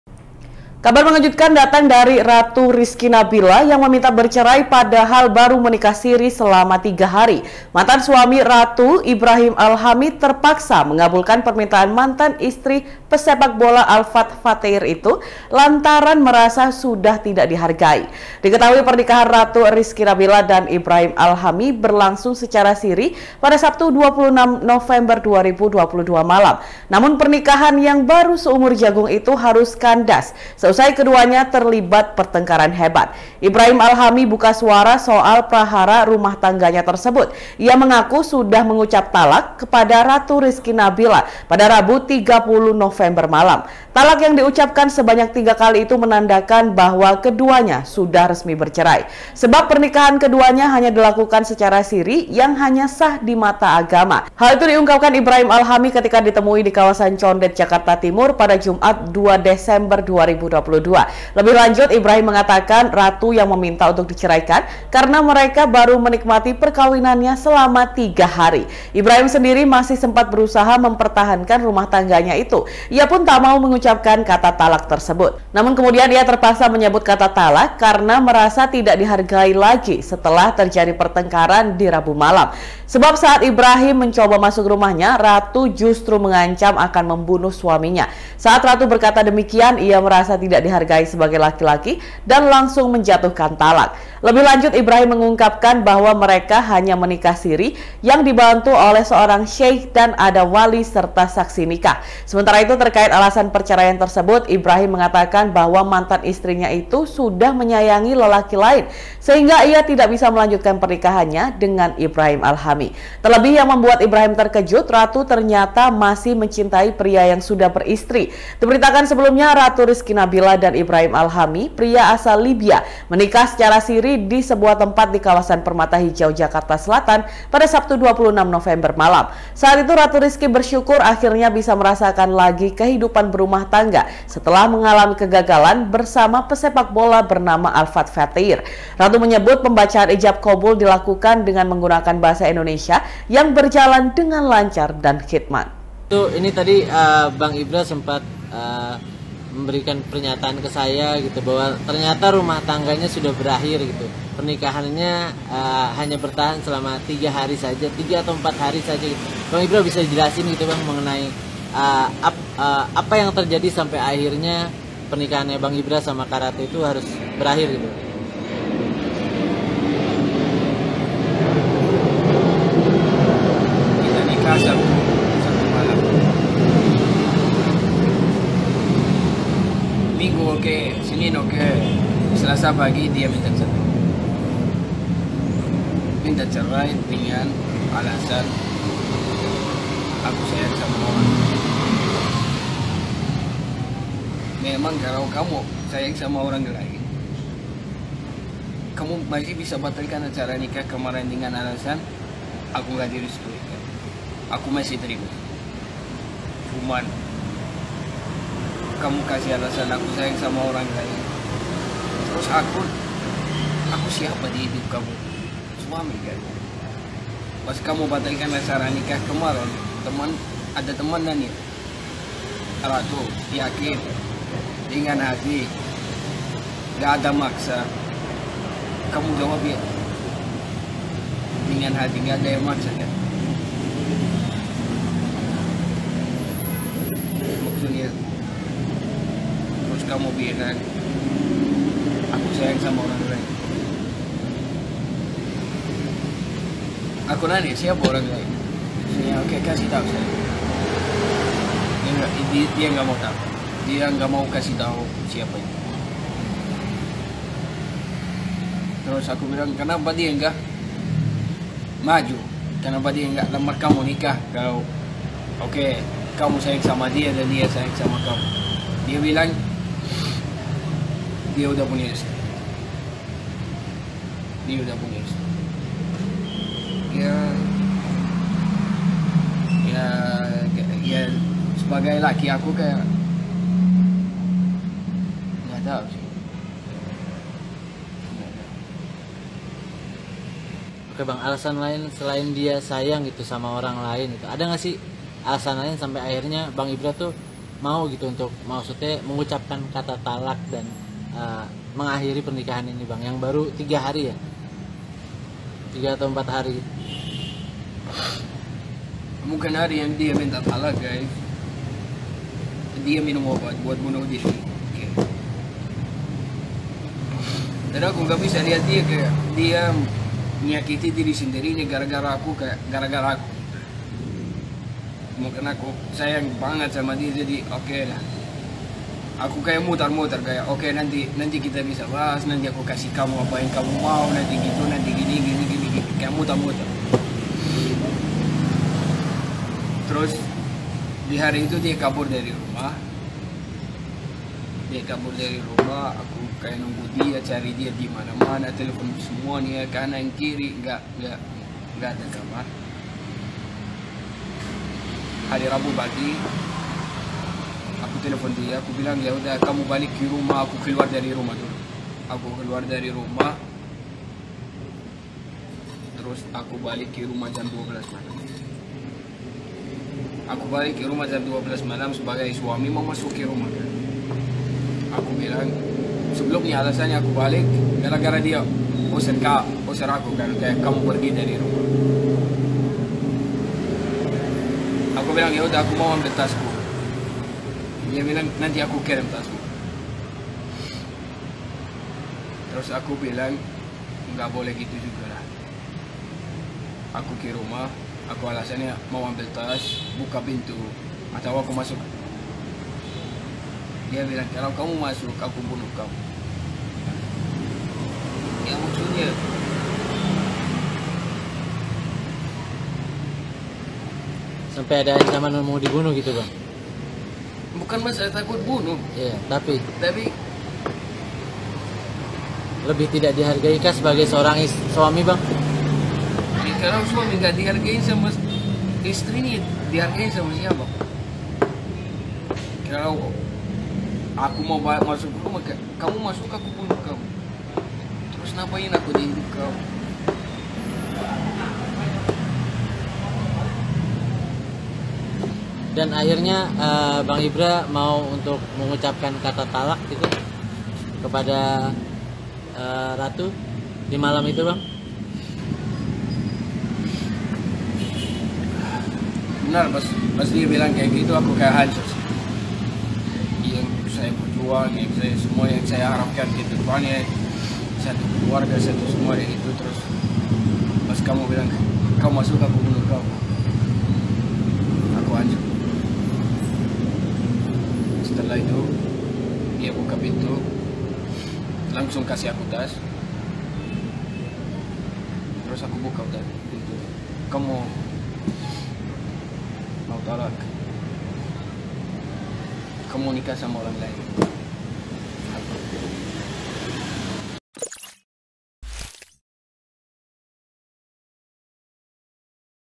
Ibrahim Ungkap Alasan Ratu Rizky Nabila Minta Cerai: Karena Sayang Suami Orang
Kabar mengejutkan datang dari Ratu Rizky Nabila yang meminta bercerai padahal baru menikah siri selama tiga hari.

Mantan suami Ratu, Ibrahim Alhami terpaksa mengabulkan permintaan mantan istri pesepakbola Alfath Fathier itu lantaran merasa sudah tidak dihargai.

Diketahui, pernikahan Ratu Rizky Nabila dan Ibrahim Alhami berlangsung secara siri, Sabtu (26/11/2022) malam.

Namun, pernikahan yang baru seumur jagung itu, harus kandas seusai keduanya terlibat pertengkaran hebat.

Ibrahim Alhami buka suara soal prahara rumah tangganya itu.

Ia mengaku sudah mengucap talak kepada ratu Rizky Nabila, Rabu (30/11/2022) malam.

Talak yang diucapkan sebanyak tiga kali itu menandakan bahwa keduanya sudah resmi bercerai.

Sebab, pernikahan keduanya hanya dilakukan secara siri yang hanya sah di mata agama.

Hal itu diungkapkan Ibrahim Alhami ketika ditemui di kawasan Condet, Jakarta Timur, Jumat (2/12/2022).

Lebih lanjut, Ibrahim mengatakan, Ratu yang meminta untuk diceraikan ketika mereka baru menikmati perkawinannya selama tiga hari.

Ibrahim sendiri masih sempat berusaha mempertahankan rumah tangganya itu.

Ia pun tak mau mengucapkan kata talak tersebut.

Namun, kemudian ia terpaksa menyebut kata talak karena dirinya merasa tidak dihargai oleh Ratu saat terjadi pertengkaran di Rabu malam.